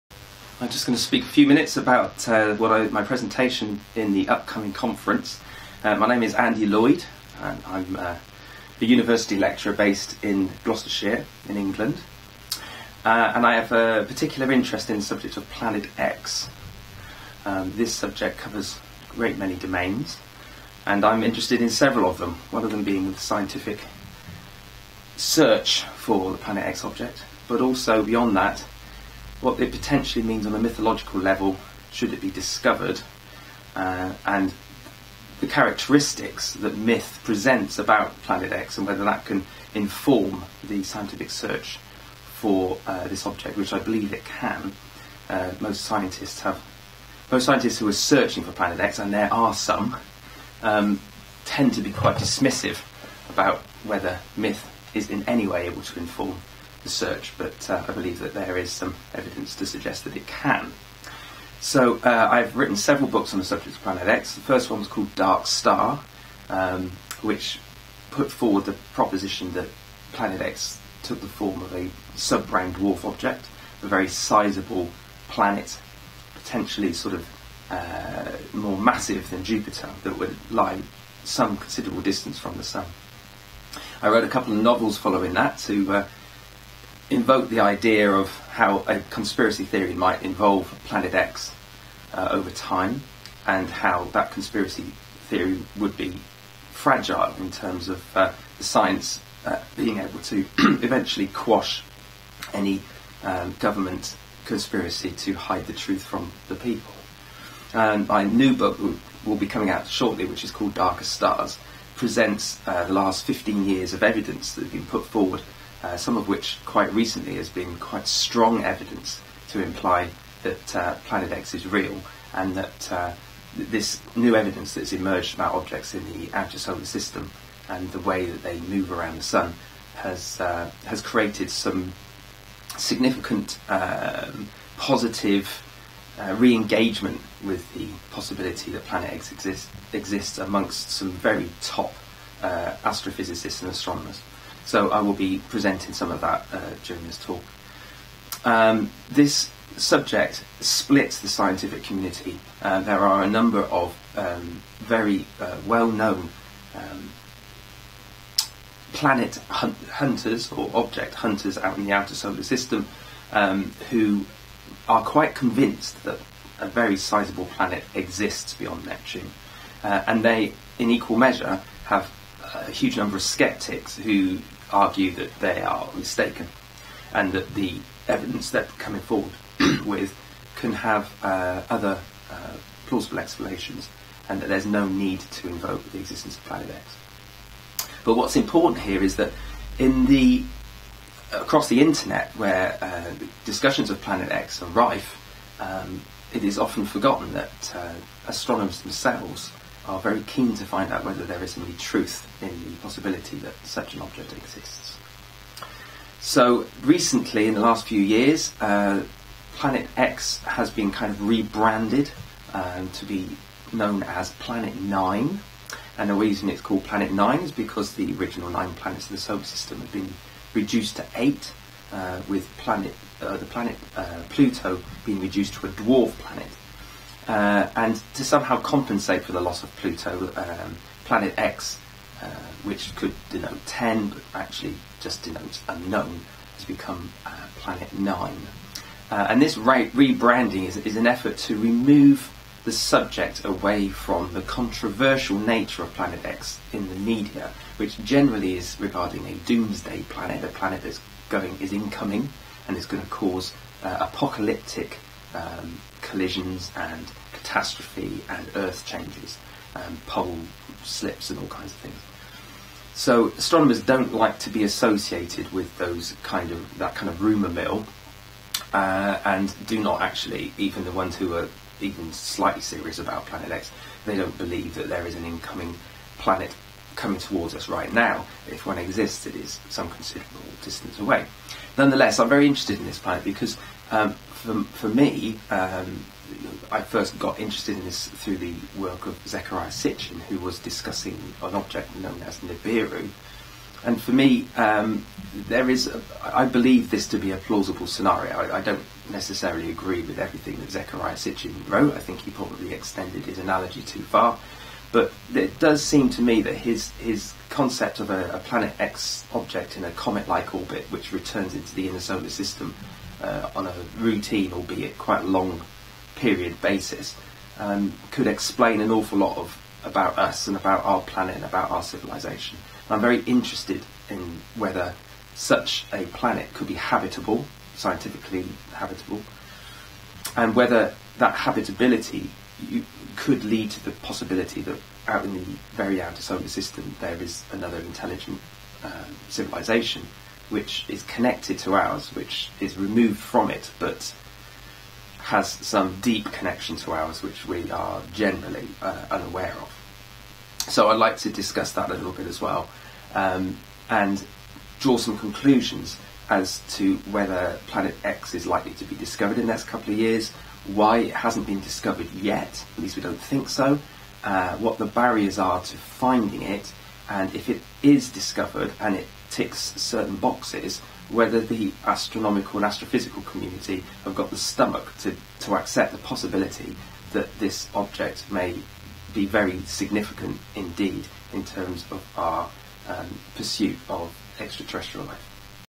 Planet X conference promo
Planet X researcher and author Andy Lloyd sets out the material he will be presenting to the upcoming Stargate to the Cosmos conference in Albuquerque, New Mexico, via web-link, 25-28 October 2018.